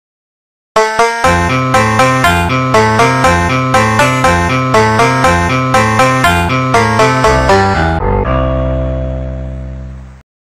Guess what song I arranged for the fanmade 2001 Leap's Phonics School (Part 2)
You won't believe what song I arranged this time for the fanmade Leapfrog Leap's Phonics School toy from 2001. I made this for The Electronic Toy Archive.